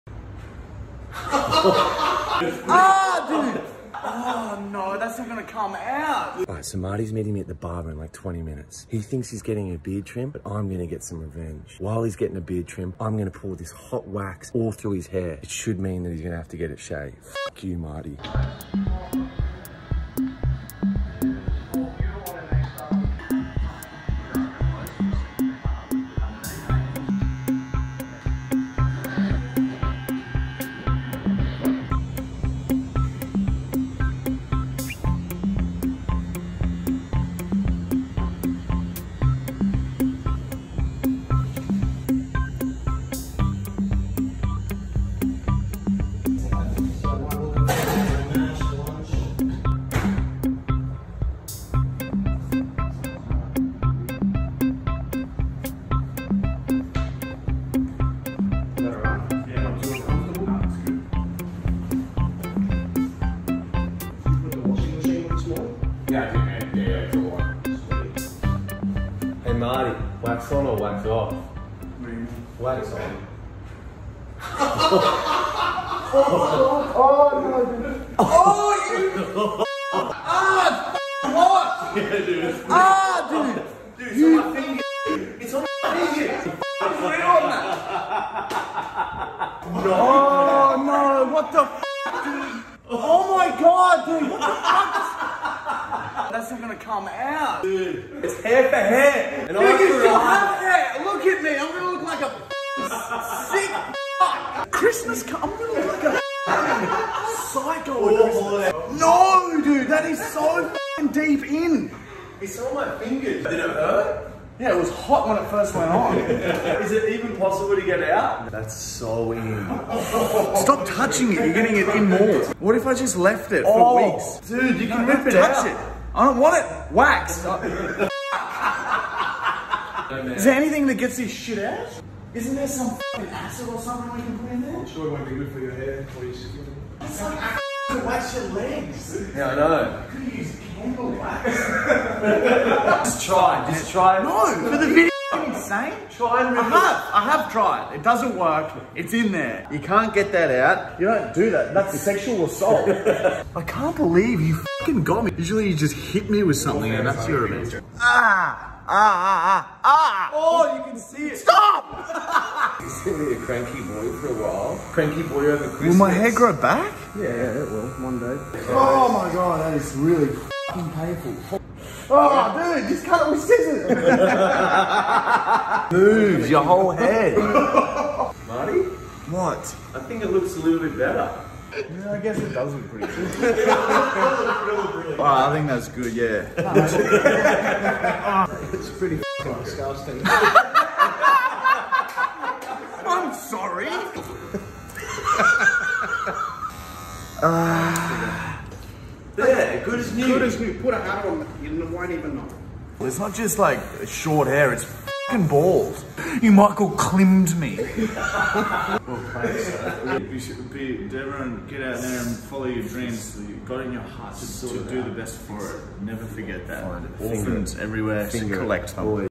Worst Hair Cut Ever Revenge!
Michael manages to get pay back on Marty for ruining his hair... by ruining his hair. A cheeky trip to the barber helps Michael disguise his evil prank, where Marty is mid beard trim when Michael pours hot wax in his hair.